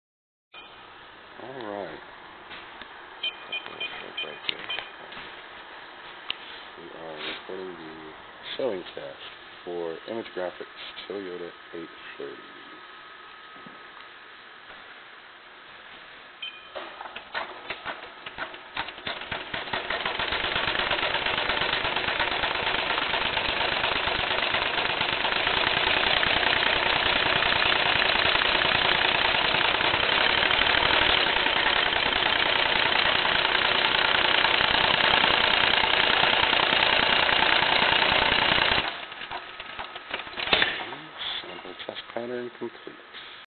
Toyota 830 Expert IG Test
successful test of circuit board repair for Toy 830 Expert embroidery machine. Machine was giving error "internal connect error". This is a common problem that we repair often.